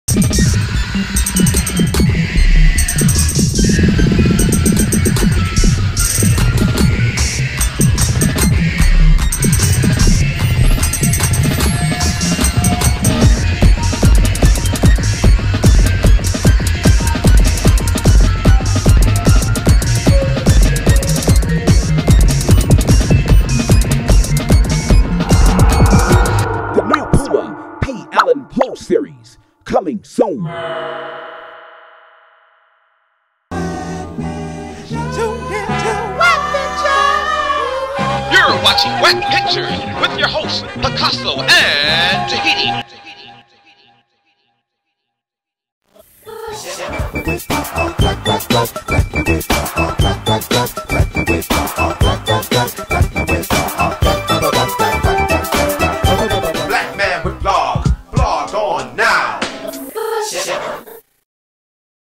PUMA Commercial [P. allen Poe Series] Coming Soon
This is a PUMA commercial we came up with just because we are some old school PUMA fanatics peep the jacket action. Plus this was shot with all photo still frames its so many that it looks like it was filmed. Photos are courtesy of the great Hal Samples and yes for those that may not know Pikahsso & Tahiti are from Dallas, Tejas the music in this comercial is by super producer Ch0

Peace
Pikahsso